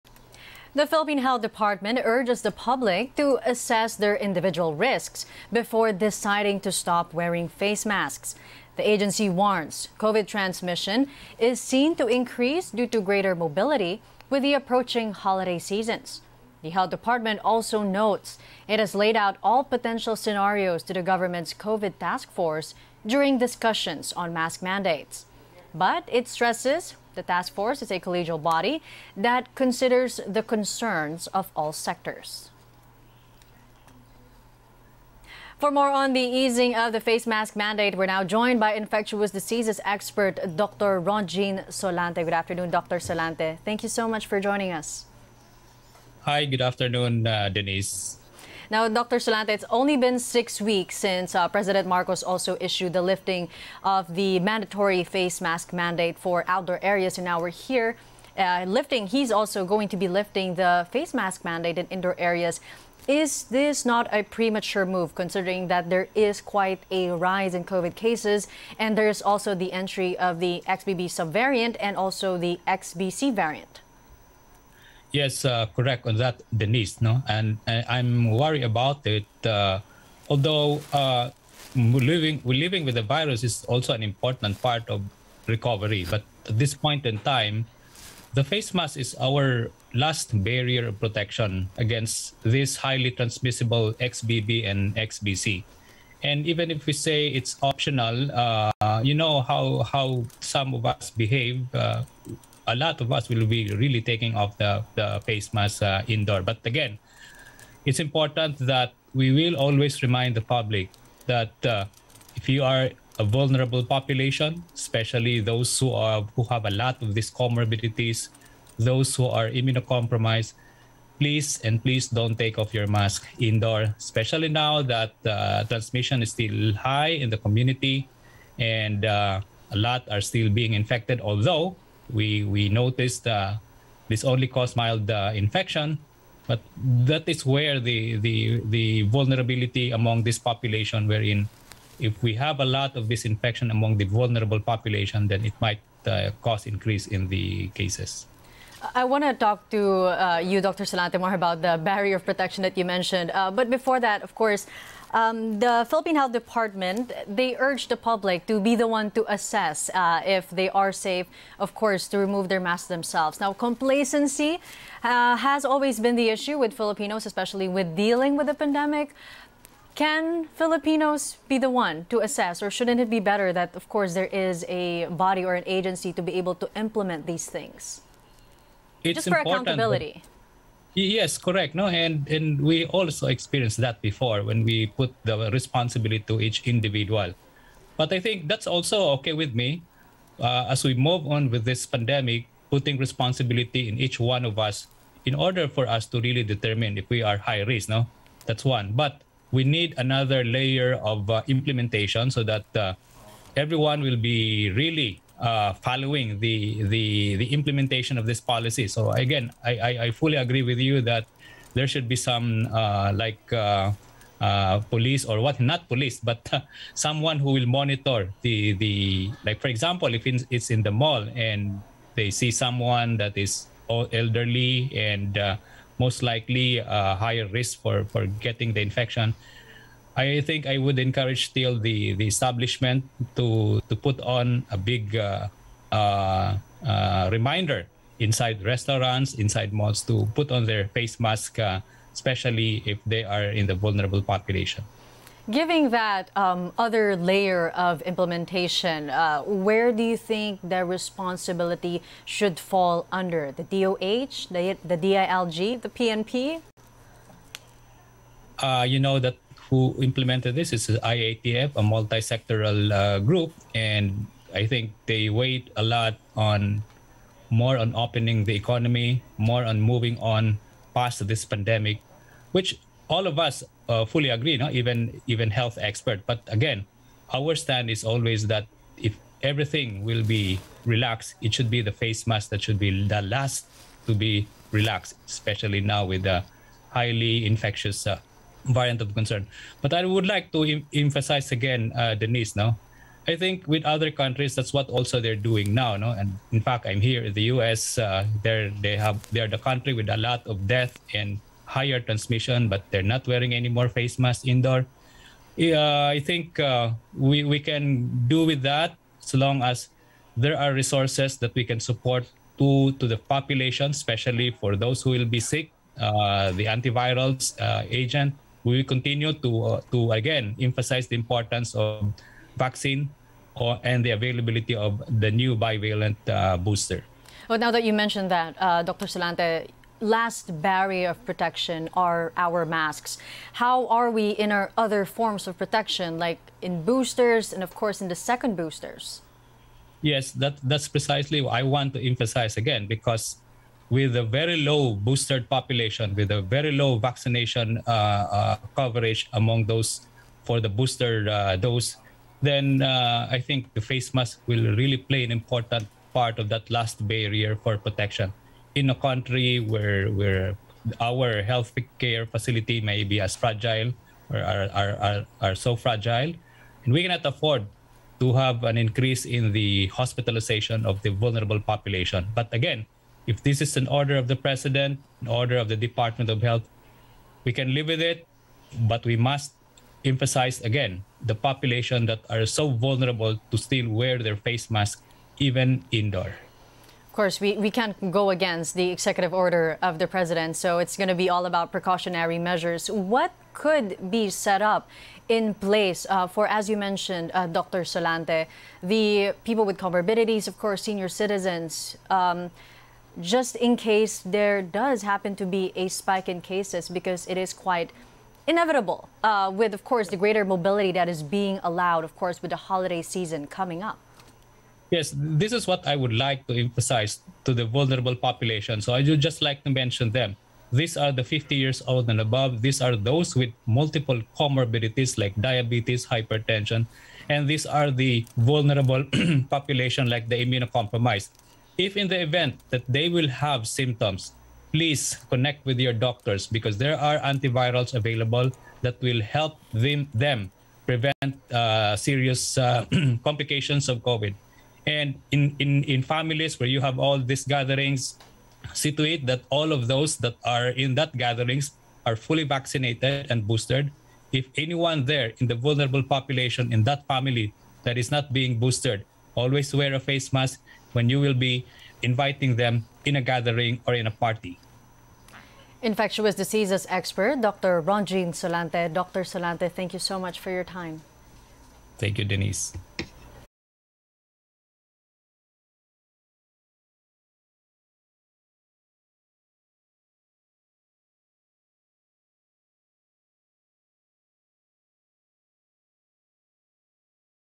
Expert: Face masks play crucial role in protecting public amid relatively low booster coverage | ANC
Top Story: Infectious Diseases Expert Dr. Rontgene Solante reacts on PH government's decision to ease the face mask mandate.